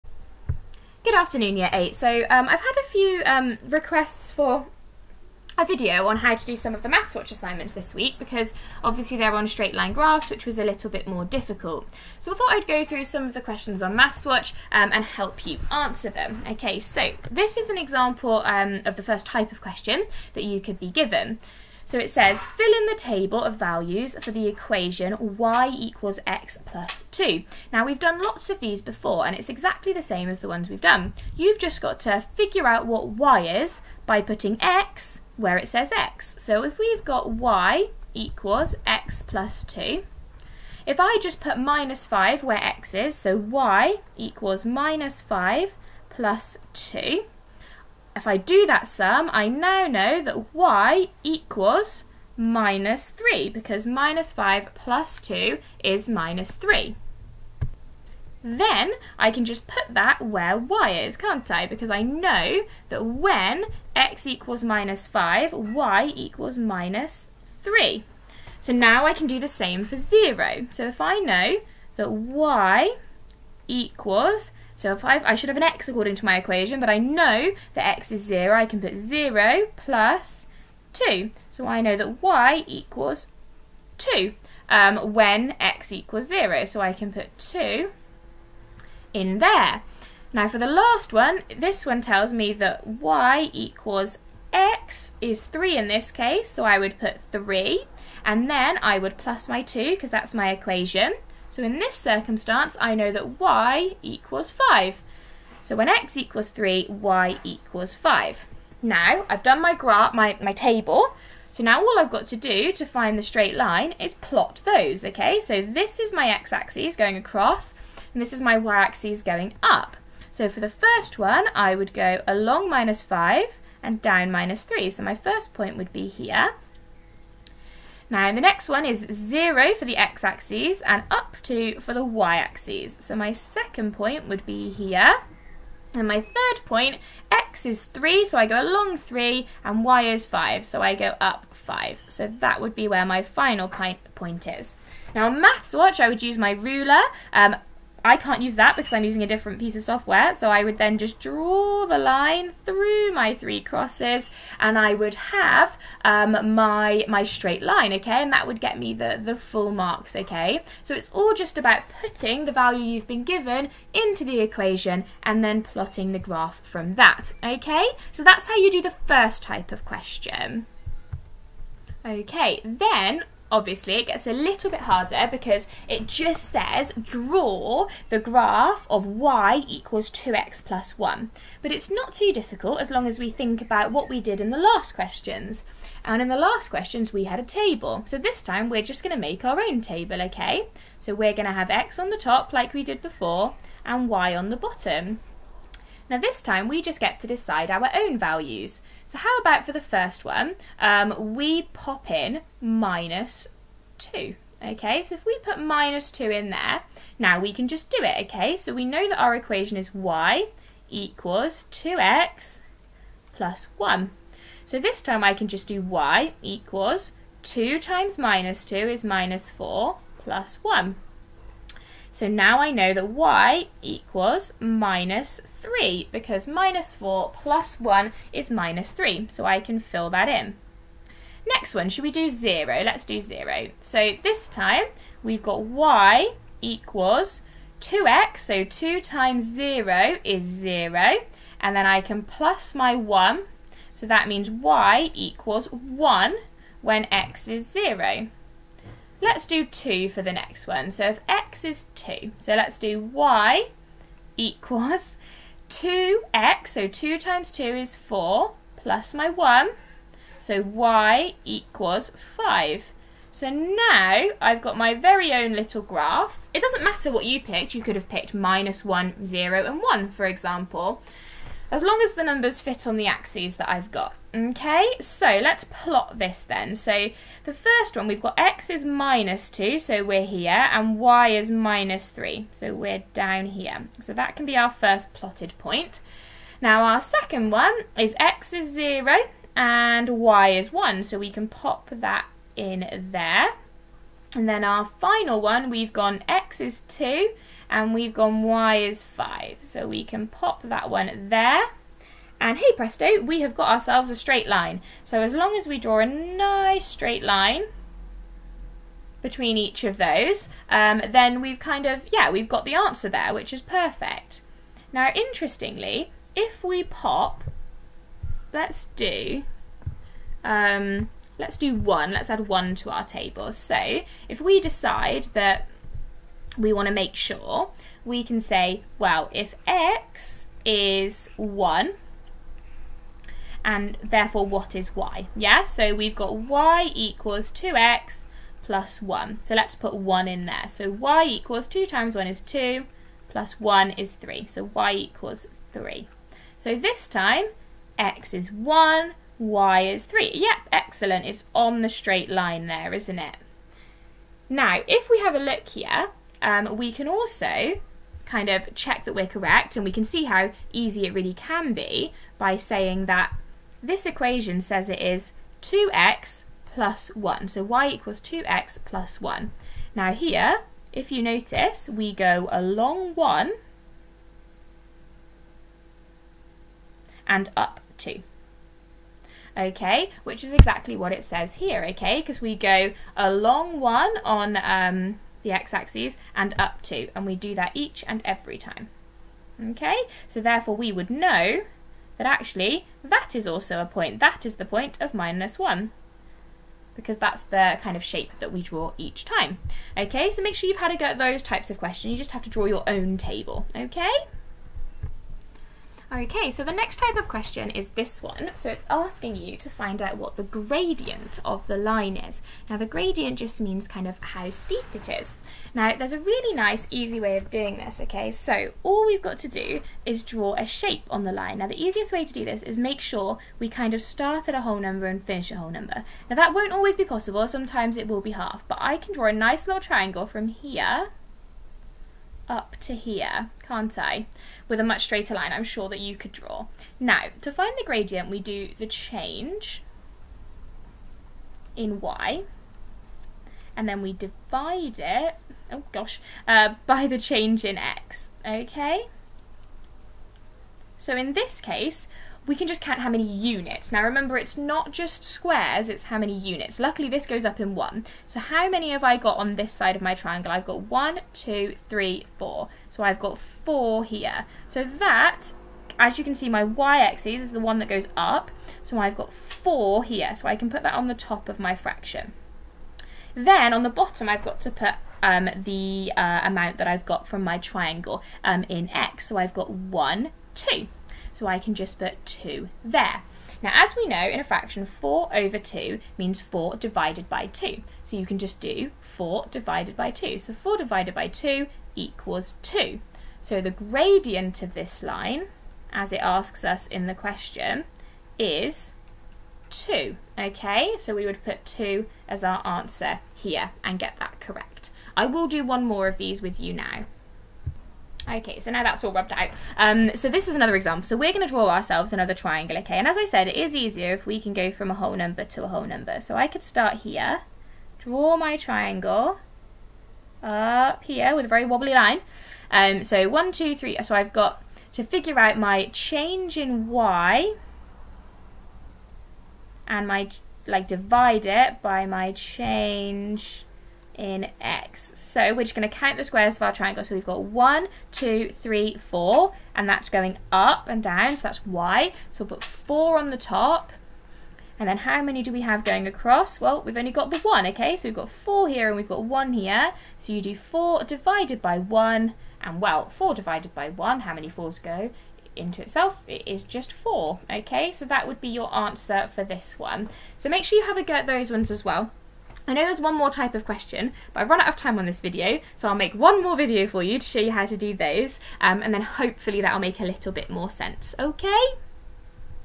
Straight Line Graphs On MathsWatch Part One
For anyone struggling with straight line graphs! There is also part two!